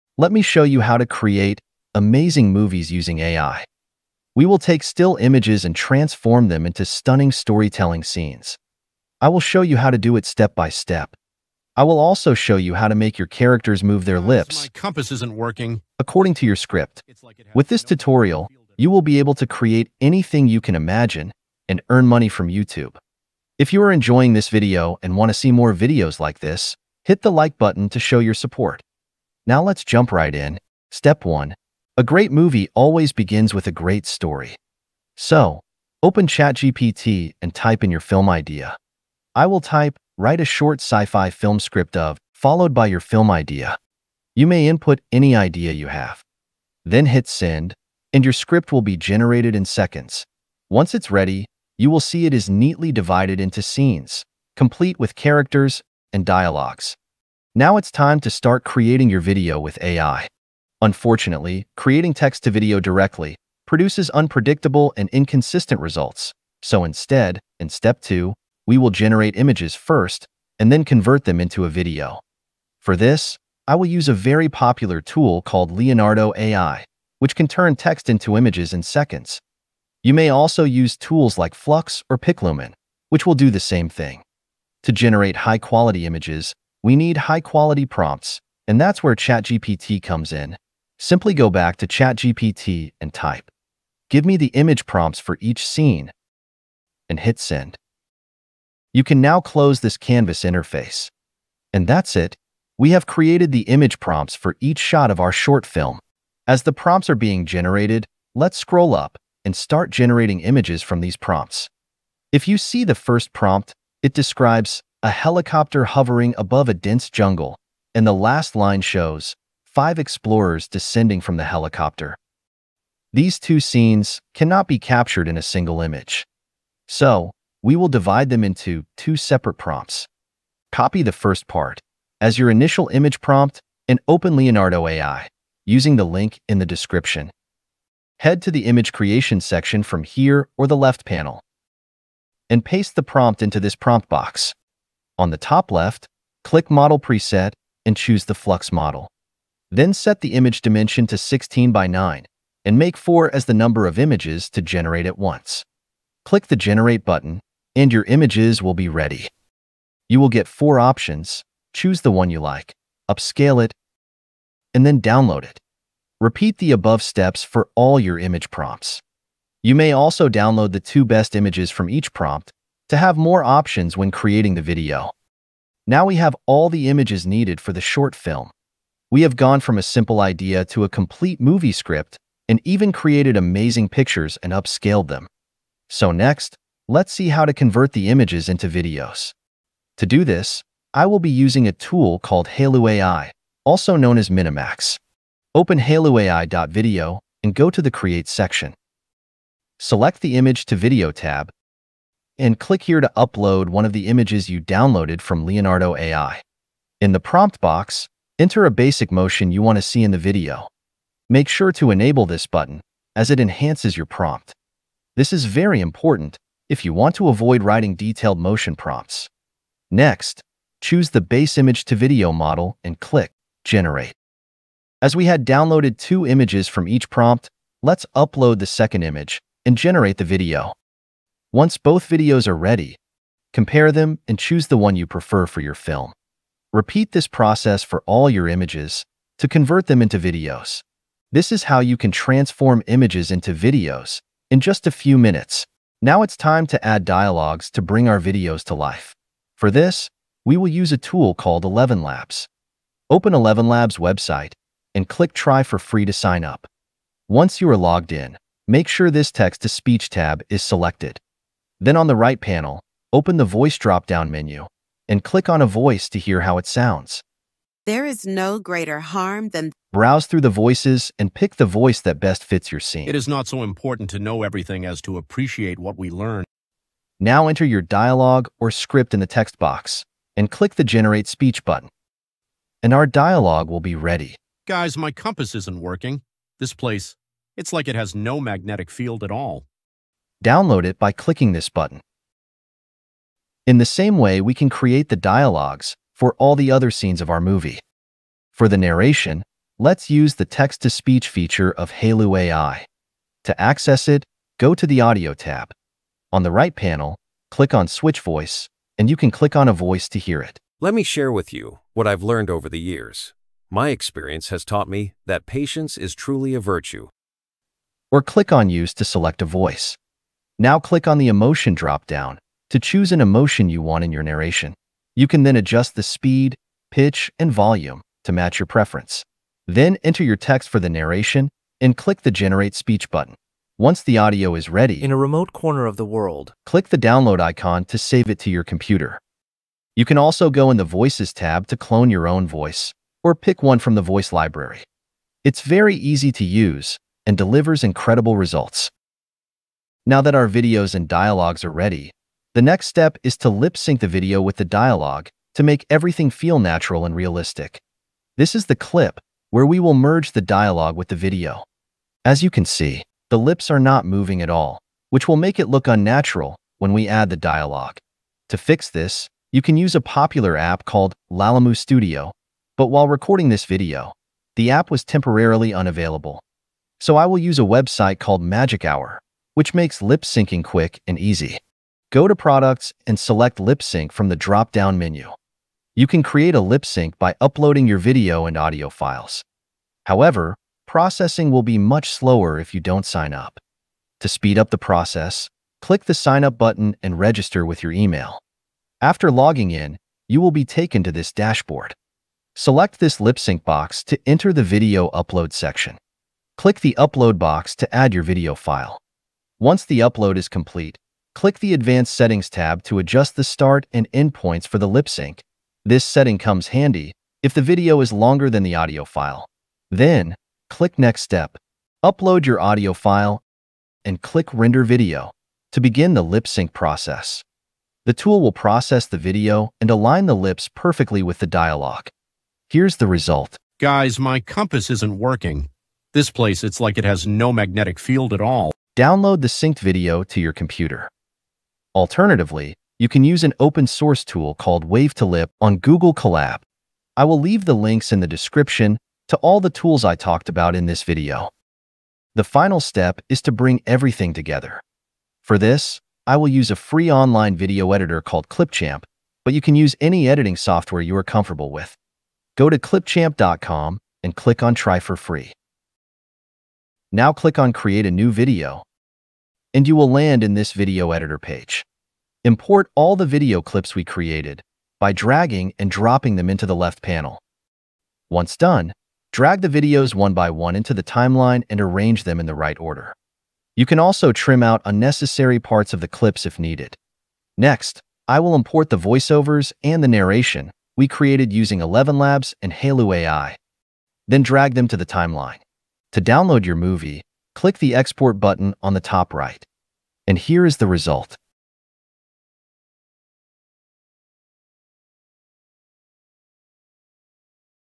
Best AI Video Generator For Text To Video and Image To Video | Create ENTIRE Film with AI
Create an ENTIRE AI Film from scratch, step-by-step tutorial.

🛠 Workflow Steps:

Step 1. Story and Dialogues

Step 2. AI Images

Step 3. AI Video
🎞 Hailuoai (Minimax) FREE Daily Credits

Step 4. Dialogues
🎙 ElevenLabs Start from 5 dollars a month

Step 5. Narrations
🗣 Hailuoai Audio FREE Limited Period

Step 6. Lip-Sync

Step 7. Editing

Thank you so much for your love and support, and if you have any issues or questions, leave them in the comments.